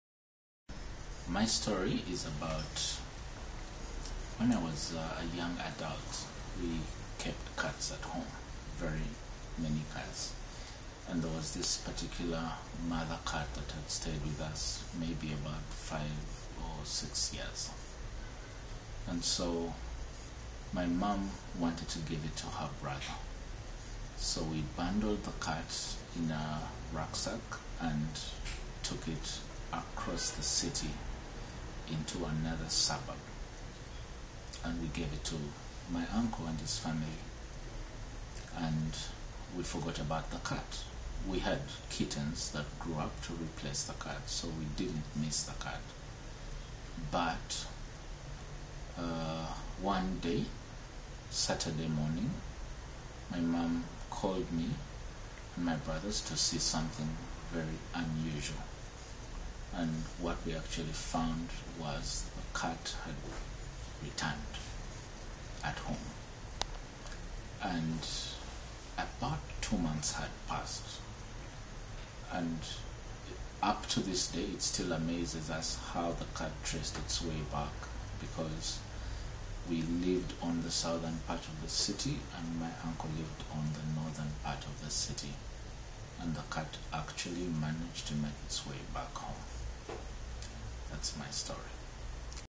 Brian Tells the Cat Story
As part of Acumen's Story Telling for Change, Lab 1 focuses on a little story from any of your previous life experiences.